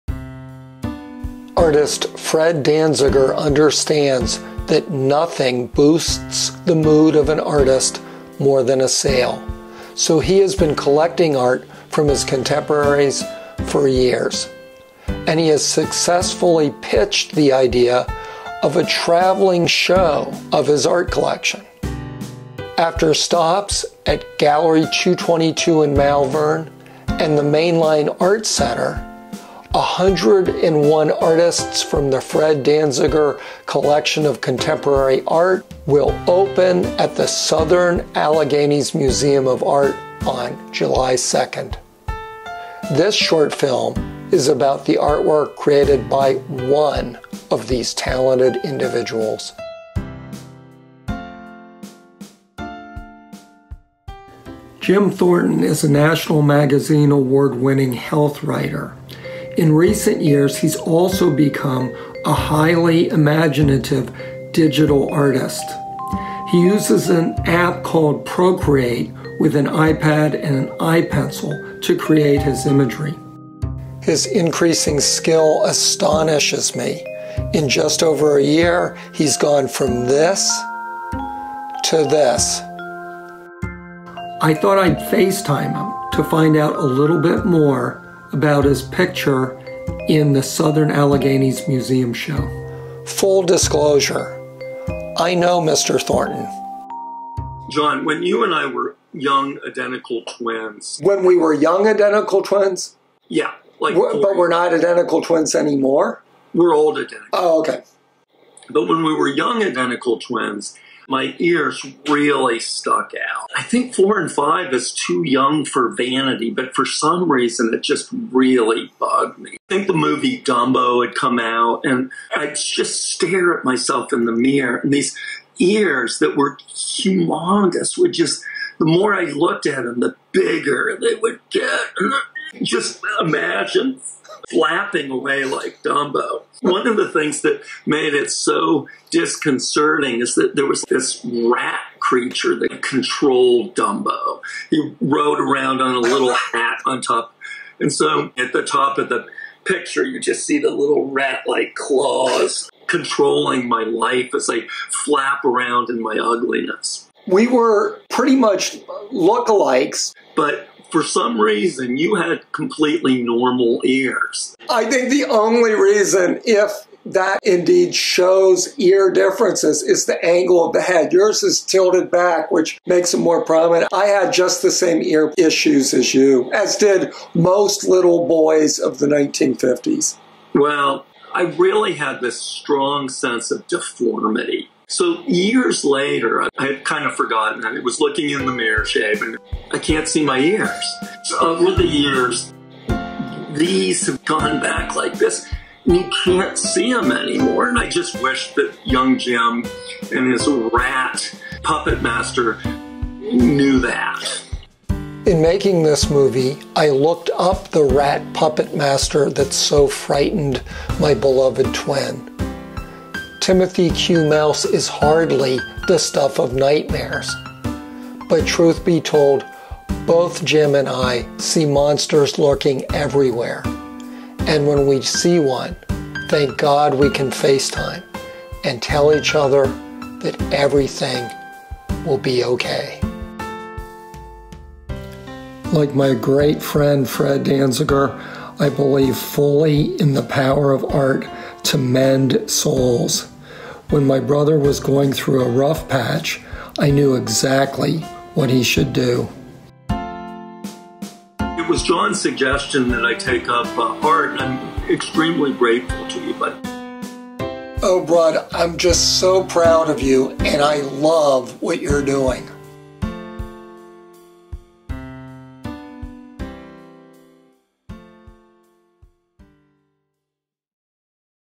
Jim Thornton's 1st Museum Show!!!
Artist Jim Thornton, for the first time in his career, has a piece in a museum show. It's a narrative image made with the Procreate app. Jim Thornton gives the backstory about the childhood neurosis that led to his painting.